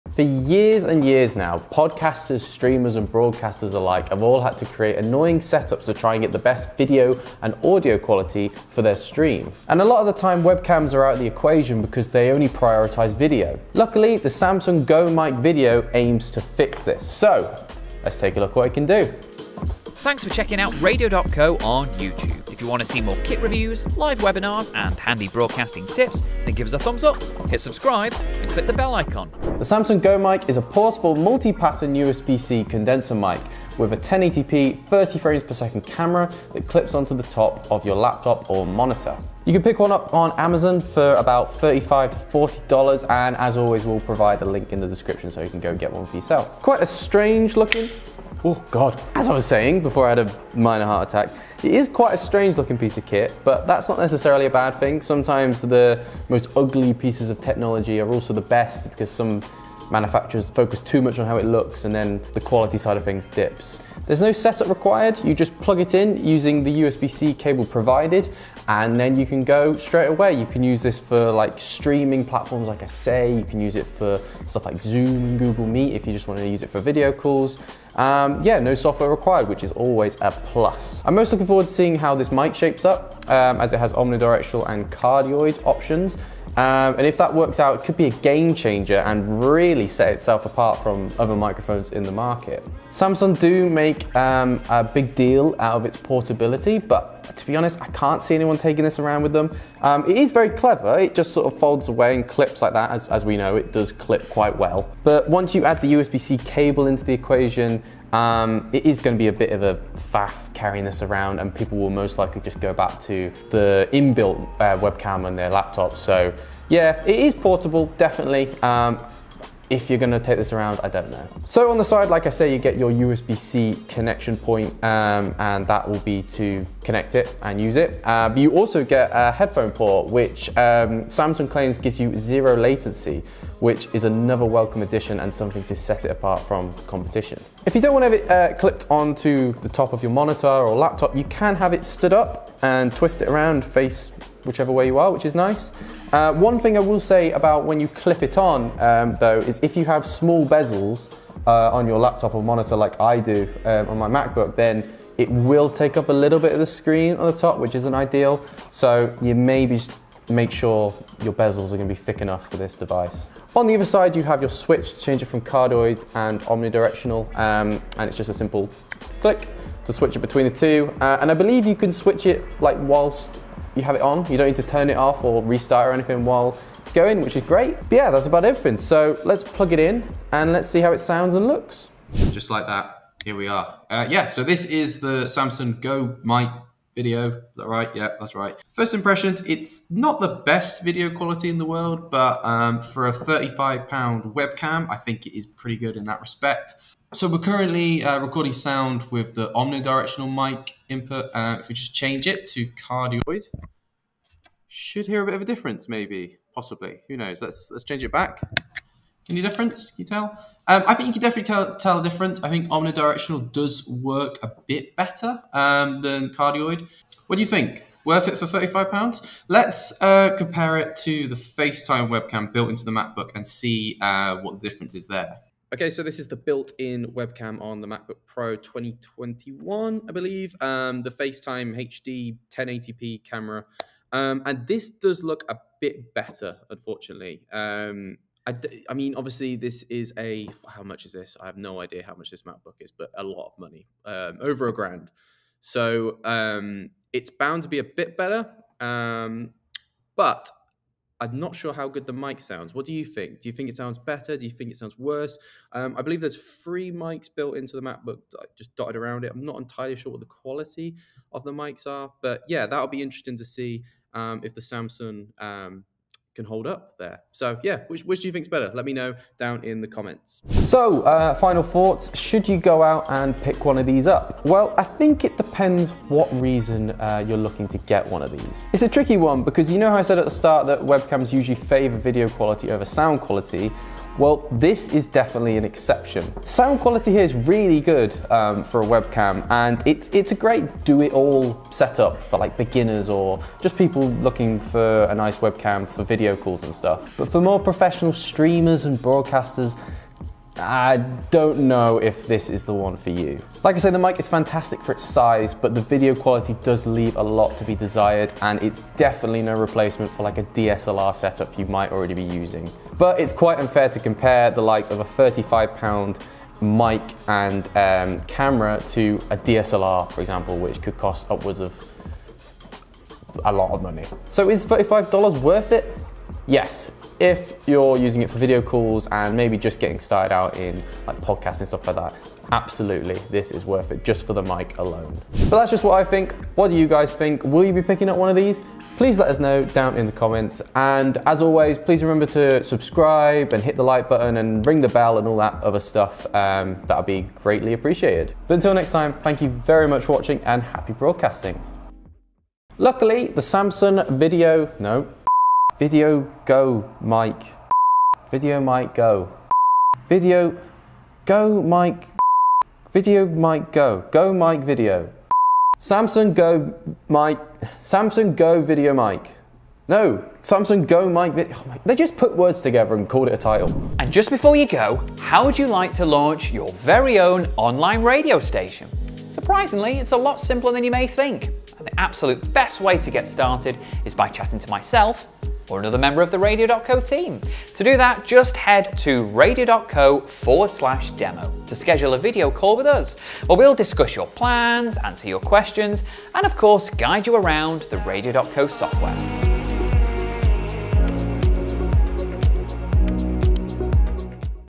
This Streaming Webcam Was Unexpected!! | Samson Go Mic Video Review
No one likes clutter. The Samson Go Mic Video aims to replace complicated streaming setups with a 'one-size-fits-all' mic and video camera. All for $95, but does it deliver? Let's find out!

CORRECTION: The Go Mic Video is $95, not $35 as stated in the video. Links below on where to buy 👇

SAMSON GO MIC VIDEO 📹

TIME CODES ⏳
00:00 - Intro
00:30 - Features & Specs
03:00 - Sound & Video Test
04:30 - Worth it?
05:35 - Outro